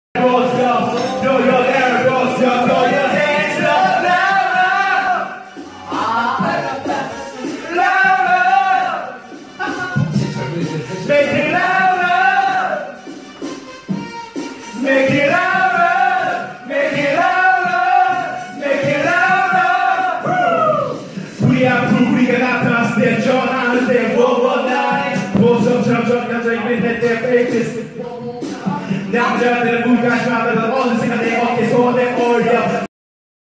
제주도 게스트하우스(게하) 광란의 밤. 코로나 밉다!
코로나오기전 제주도에서의 게스트하우스. 넘나 즐거웠다~~!